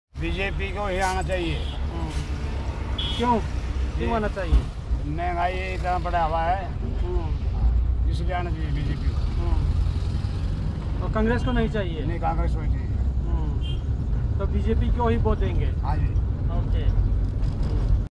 BJP Manipur-Close encounter with a Taxi driver
A taxi driver in Delhi wants BJP